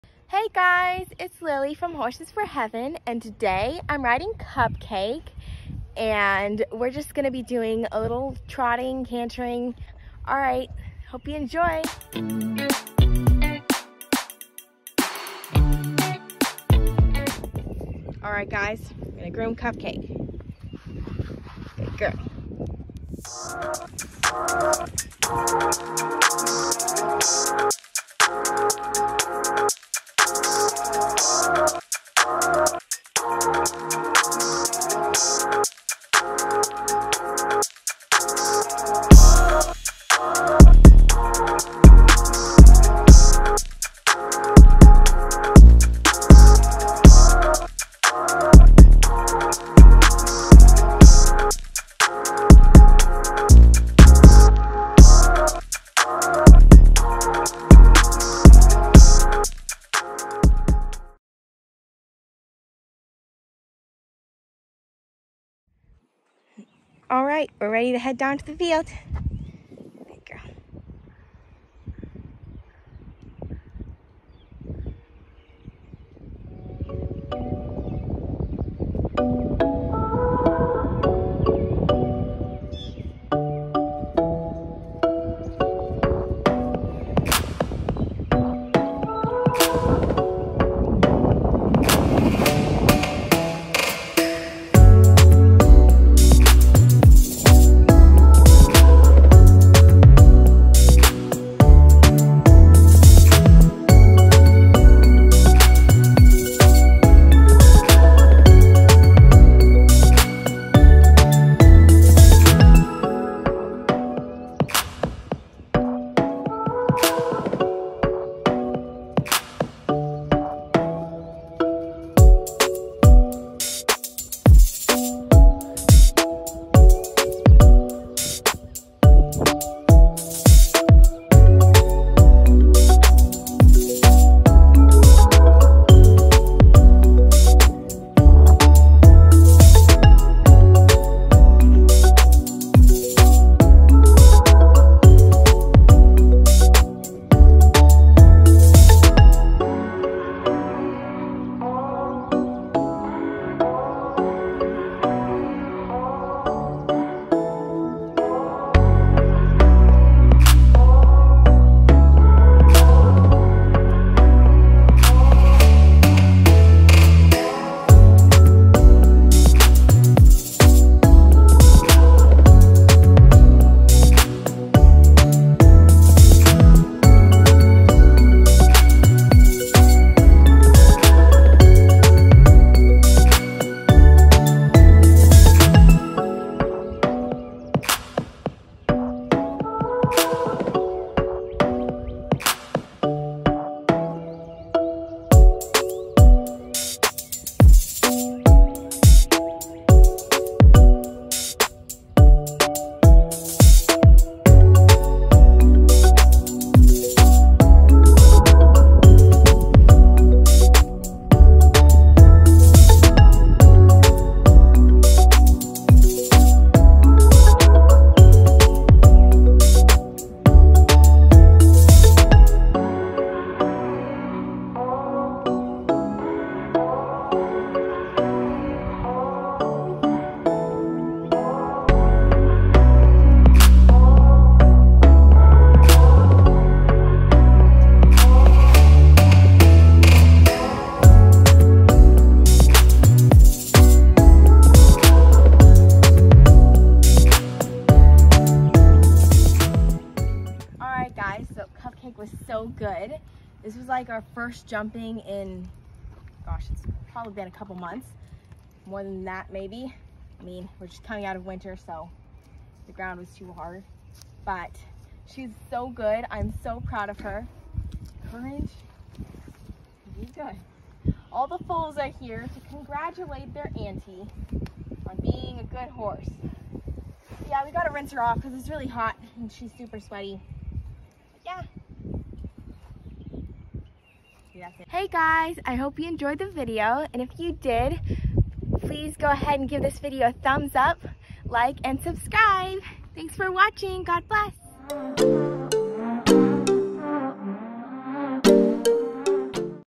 Jumping our pony for the first time in months!
Hey guys, its Horse for Heaven! Today, Lily is jumping Cupcake for first time in months! Will Cupcake behave?🐴⁉️Watch until the end to find out! We hope you enjoy 😀!